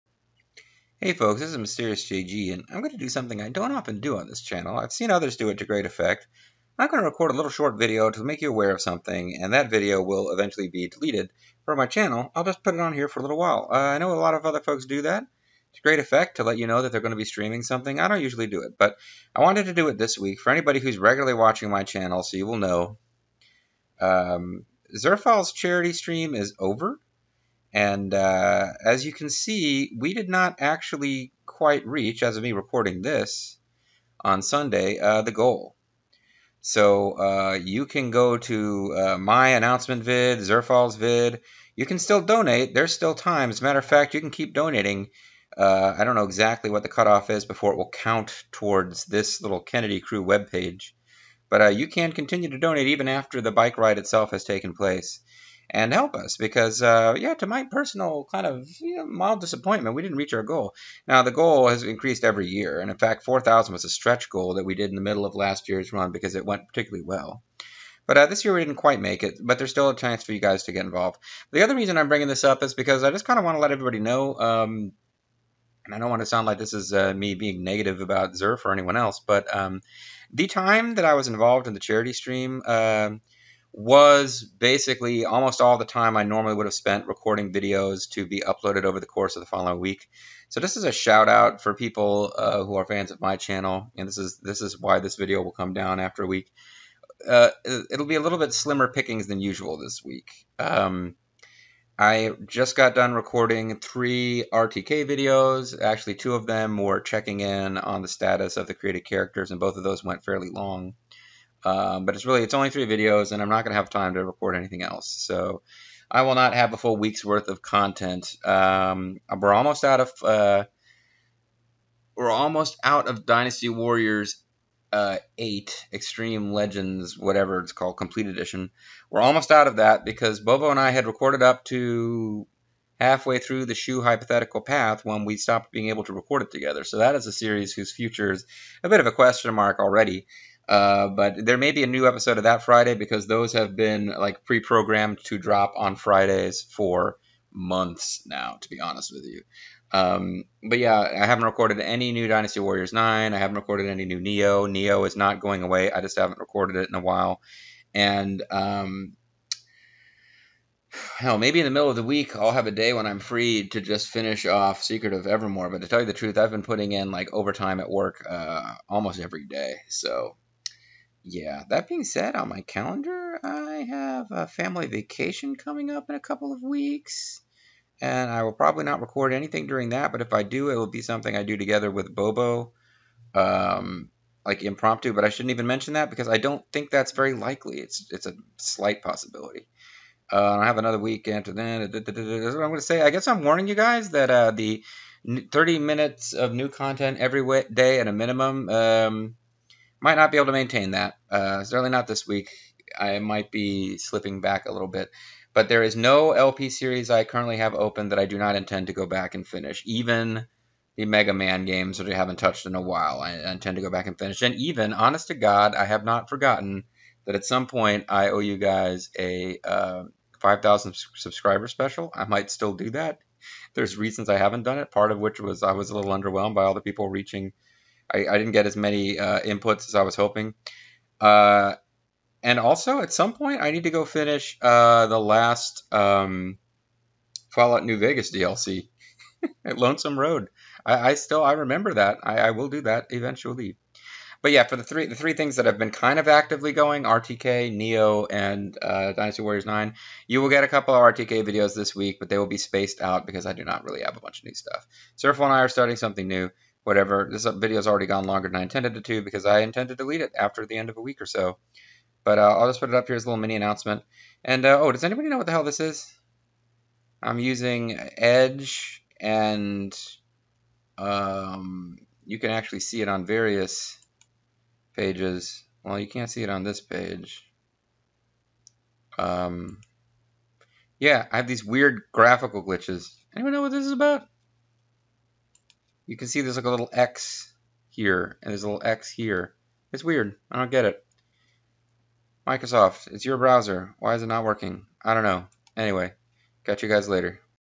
Mini Announcement - Zerf Charity Donations Still Open and Slim Upload Pickings This Week
As the title says, you can still donate to Zerf's charity bike ride.  And in quasi-related news, I won't have a lot of uploads this week.